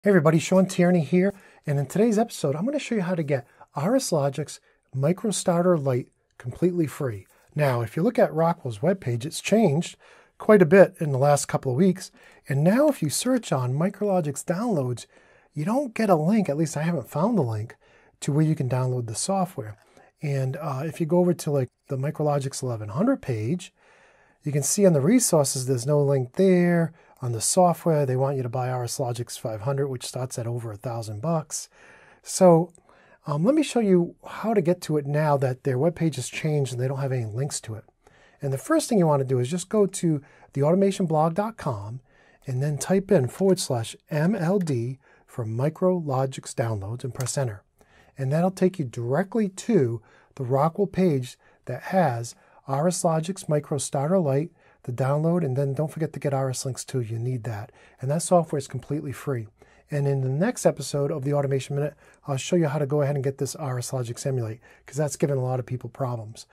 How to download RSLogix & RSLinx for free! (link to new procedure in description)
Shawn walks through where to download RSLogix and RSLinx for free in 2016 in Episode 36 of The Automation Minute Season 2.